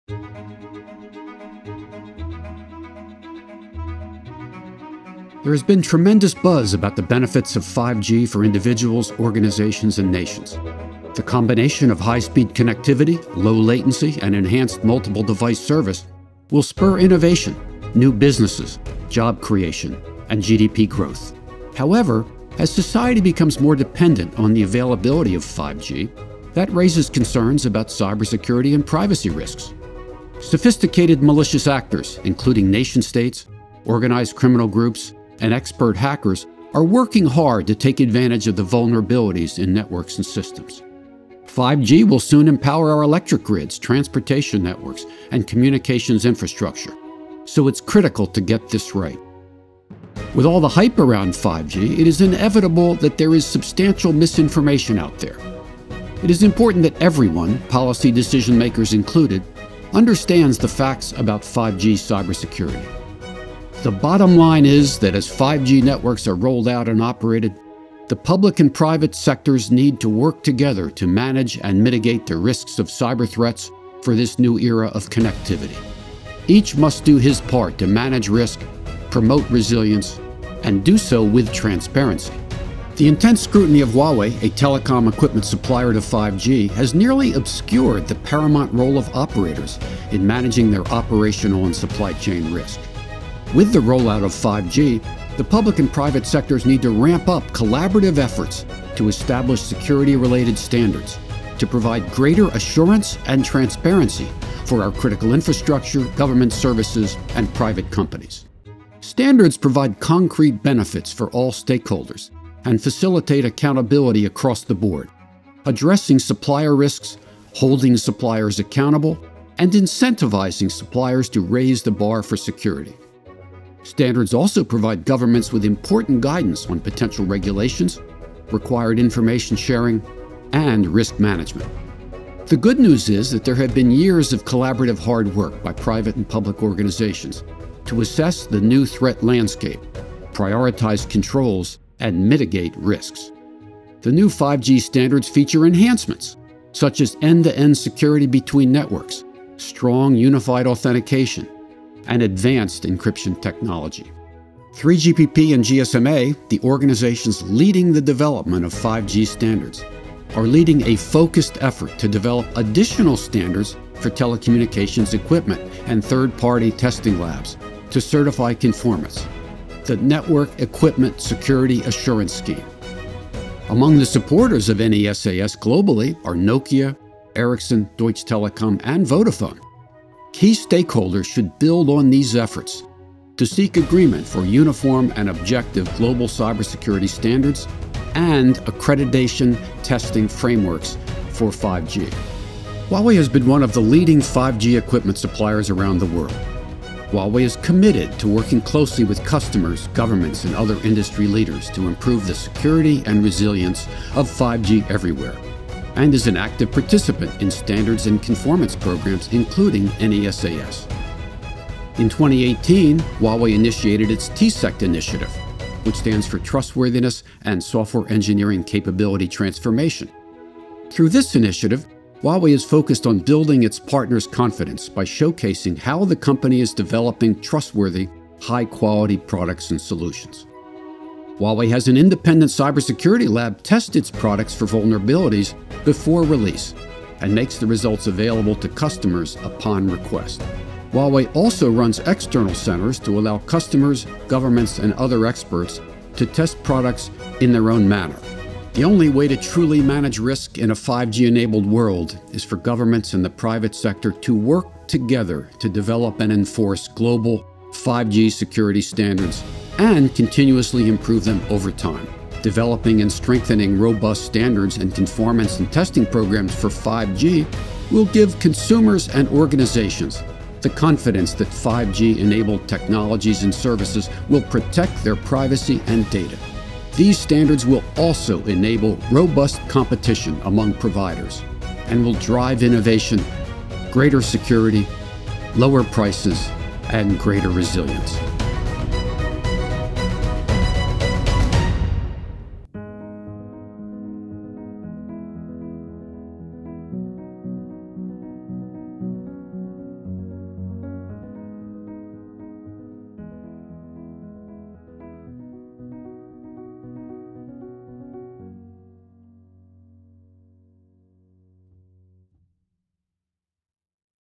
5G Security: A Shared Responsibility
5G security — we all have a part to play. It’s not about assigning responsibility; it’s a shared responsibility. Huawei USA Chief Security Officer Andy Purdy tells us why.